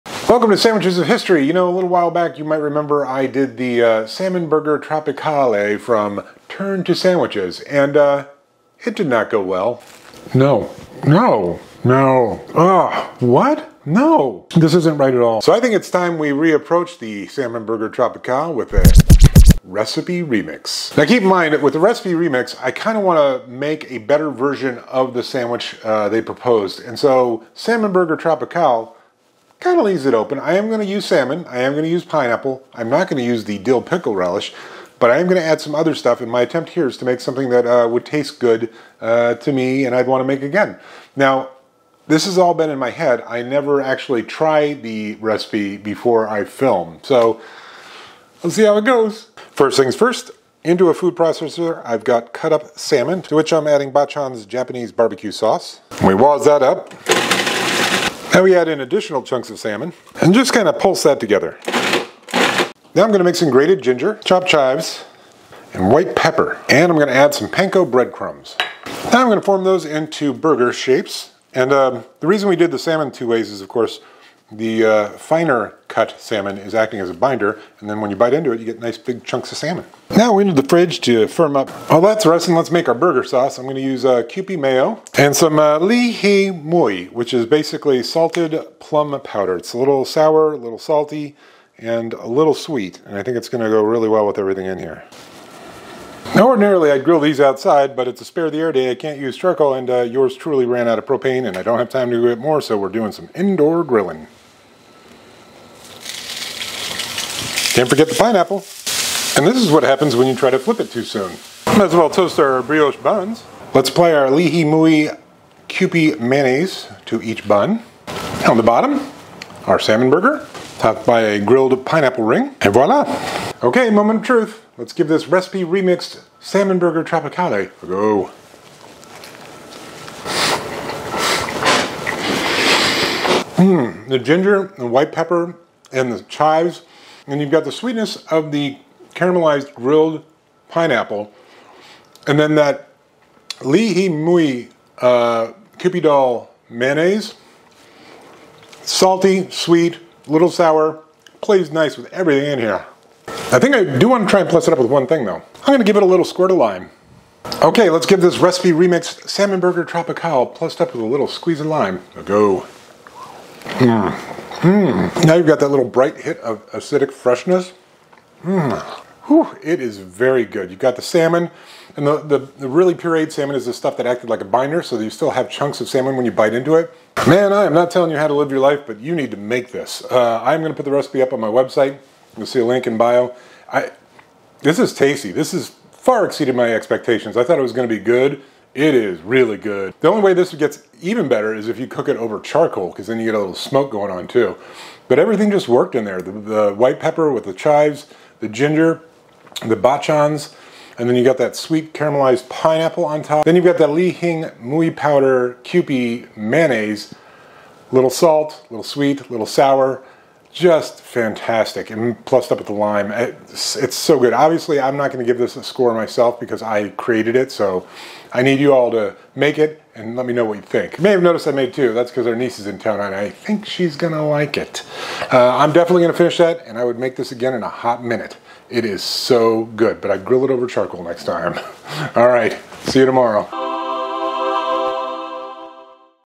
Recipe Remix! Salmonburger Tropicale Sandwich (1957) on Sandwiches of History
When last we saw the Salmonburger Tropicale from 1957’s Turn To Sandwiches of 1957 it did not go well. The recipe called for dill pickle relish which seemed to clash with almost everything else. Well, today I’m giving the Salmonburger Tropicale the Recipe Remix treatment. While I do want to pay off the name, I do not require myself to use all the same ingredients. Otherwise it would just be a Recipe Repeat. And frankly, I think we can do better.

Make the Recipe Remixed: Salmonburger Tropicale!

Pre-order Sandwiches of History: The Cookbook!